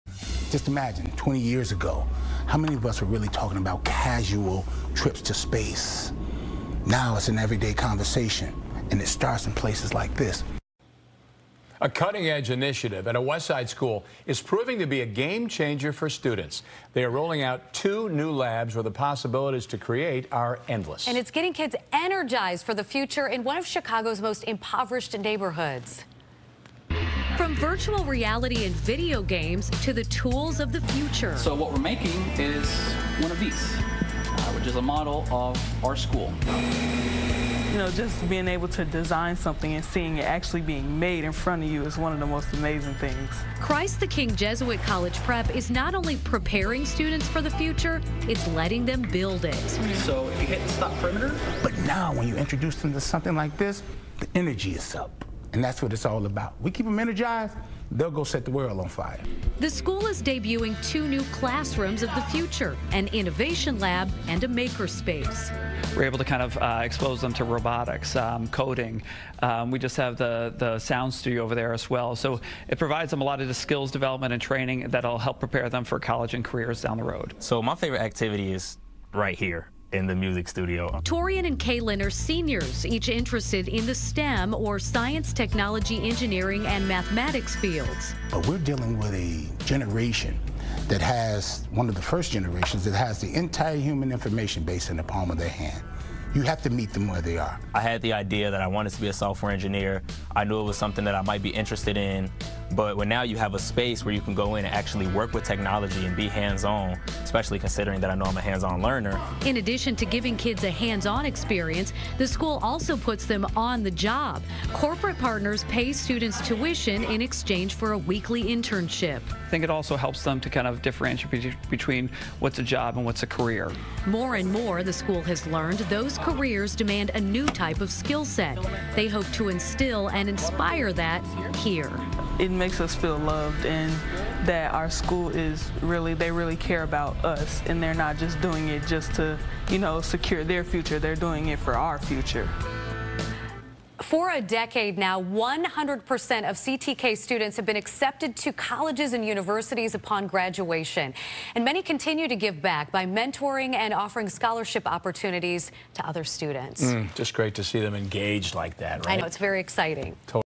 Chicago school debuts classrooms of the future to prepare students for next-gen jobs
A cutting edge initiative at a school on Chicago's West Side is proving to be a game changer for students after rolling out two new labs where the possibilities to create are endless.